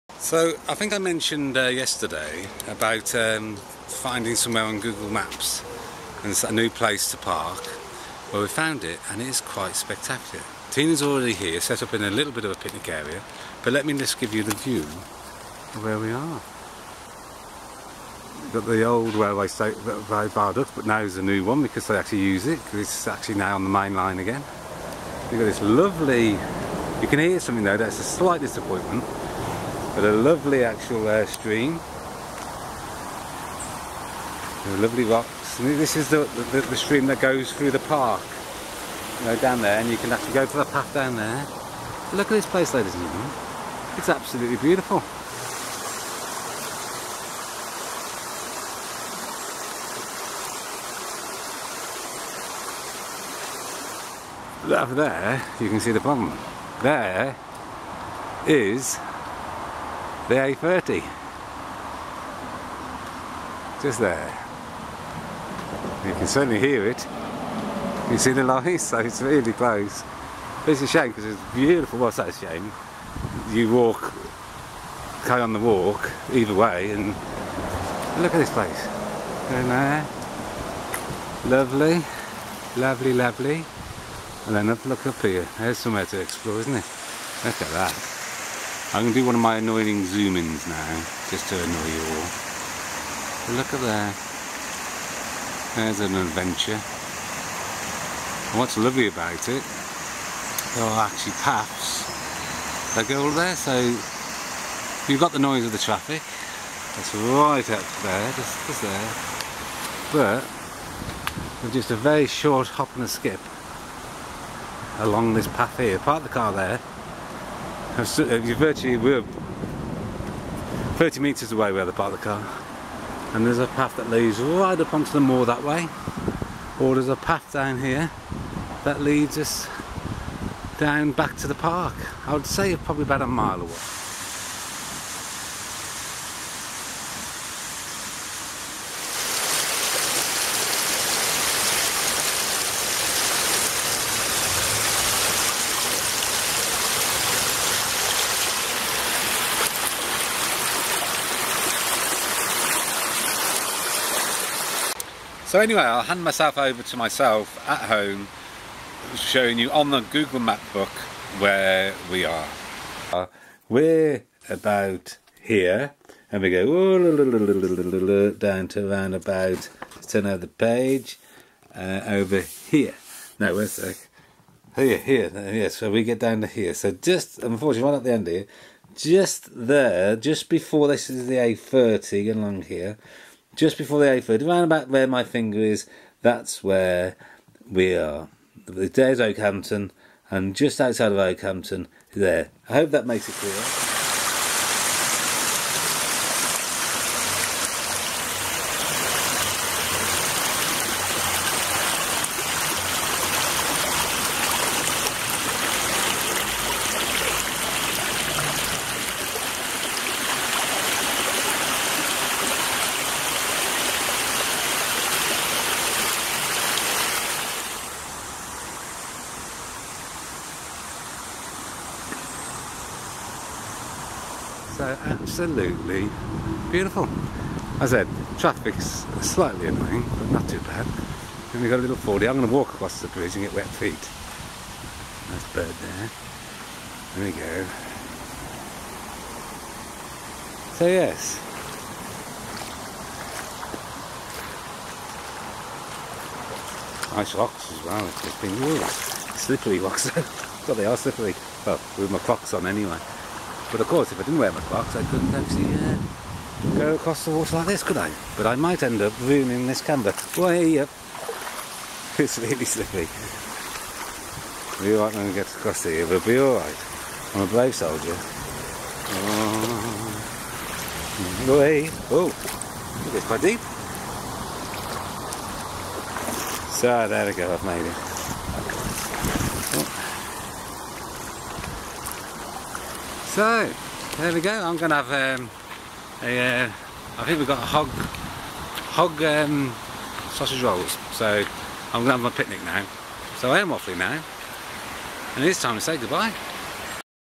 BBB Paths along East Okement river, Ball Hall Viaduct Okehampton Devon
A lovely area with walks along East Okement River just outside of Okehampton, parking for about 6 cars, we stopped today for a picnic, no picnic benches, but lovely little area next to the river under the trees.